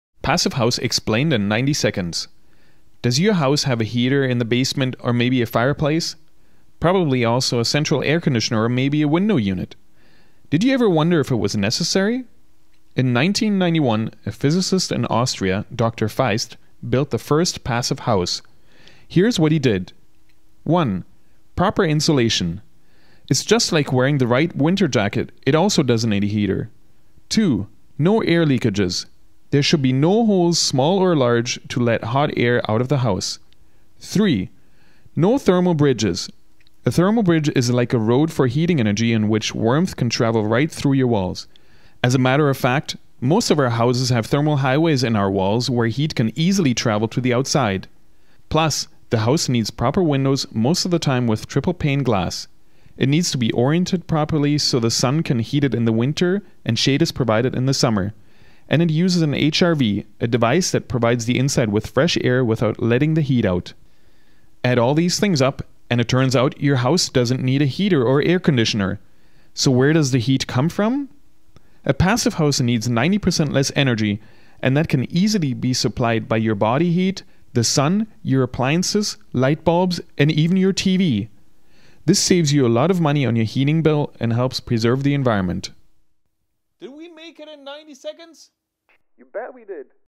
Bâtiment passif expliqué en 90 secondes (passivhaus en français)
Est-ce que votre maison a un moyen de chauffage? Vous êtes-vous demandé si c’est toujours nécessaire? Ce si est une introduction du bâtiment passif en 90 secondes. (passivhaus)